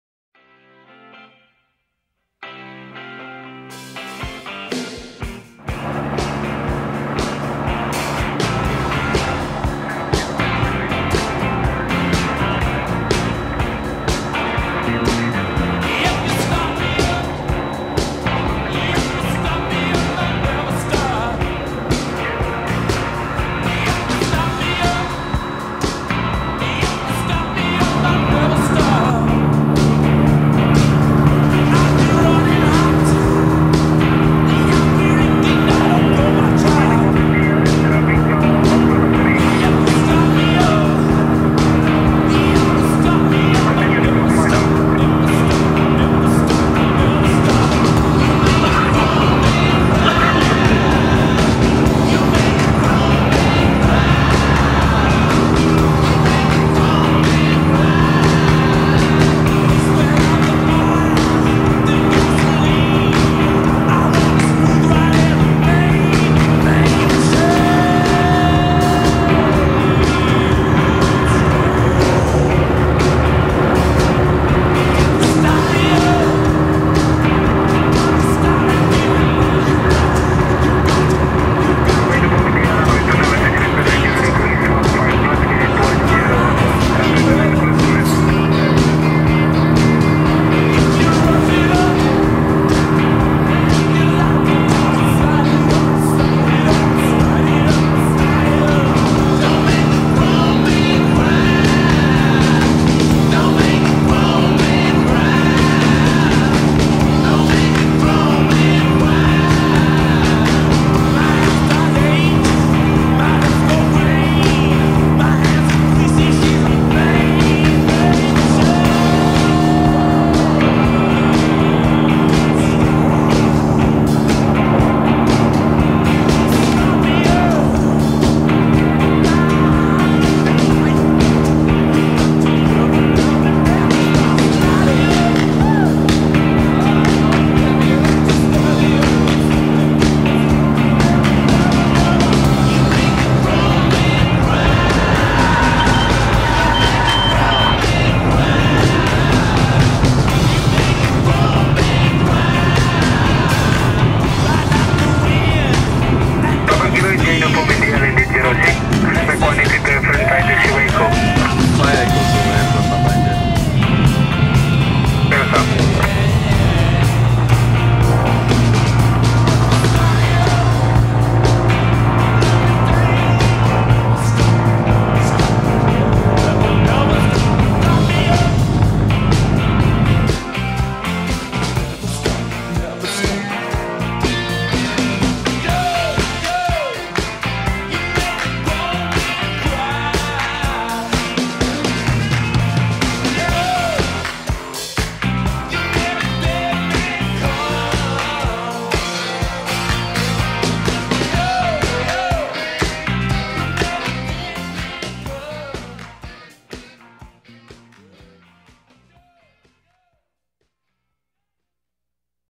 Tactical Landing And Approach (microburst)
Tactical Landing And Approach (microburst) Tecnam P2006T Bandung Pilot Academy Husein Sastranegara Airport